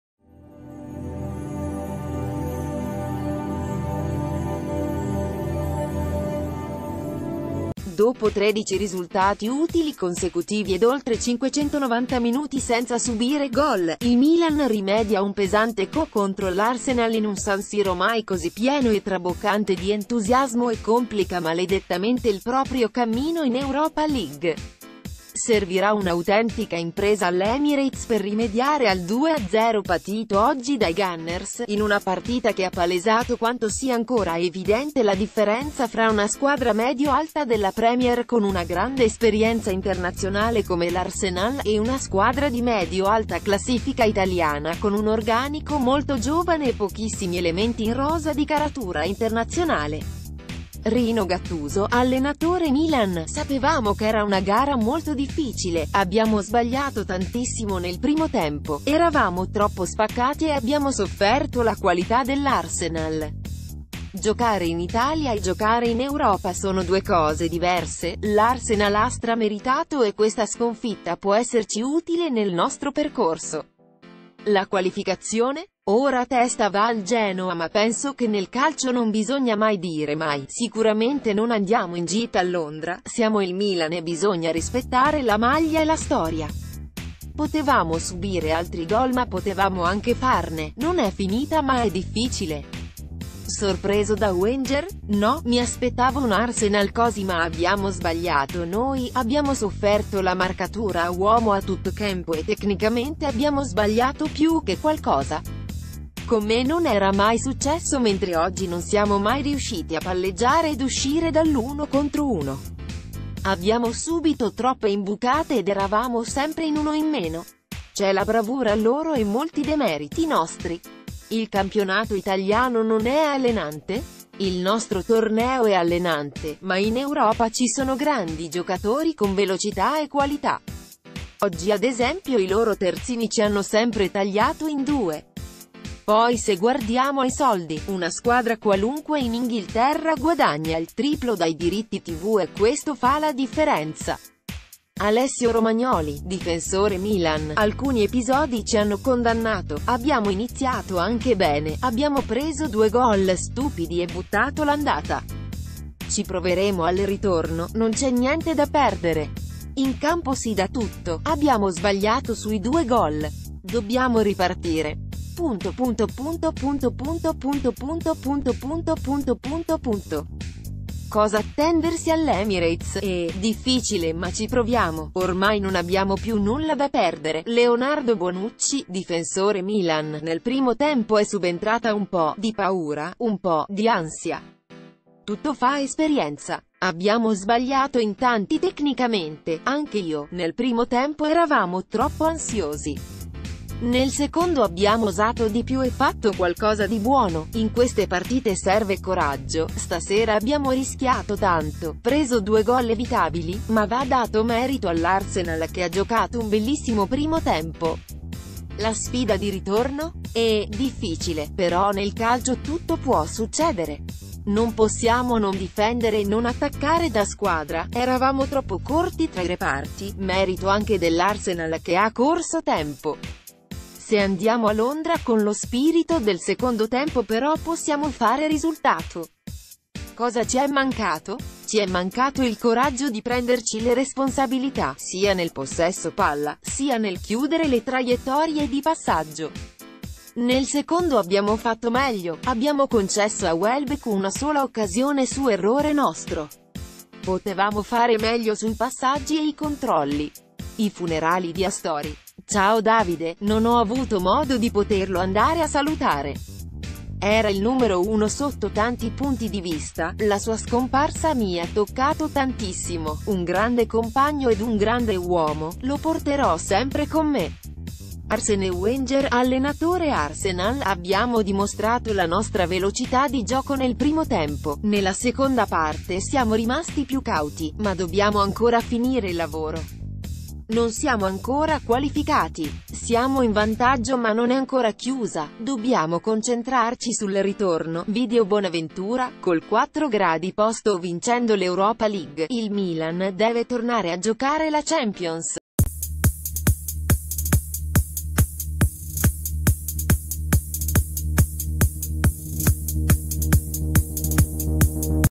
Ultime notizie | Gattuso: "Mai giocato da squadra. Ma a Londra non andiamo in gita...". Bonucci: ...
Ultime notizie | Gattuso: "Mai giocato da squadra. Ma a Londra non andiamo in gita...". Bonucci: "Mancato coraggio"

Dopo 13 risultati utili consecutivi ed oltre 590’ senza subire gol, il Milan rimedia un pesante ko contro l’Arsenal in un San Siro mai così pieno e traboccante di entusiasmo e complica maledettamente il proprio cammino in Europa League. Servirà un’autentica impresa all’Emirates per rimediare al 2-0 patito oggi dai Gunners, in una partita che ha palesato quanto sia ancora evidente la differenza fra una squadra medio alta della Premier con una grande esperienza internazionale come l’Arsenal, e una...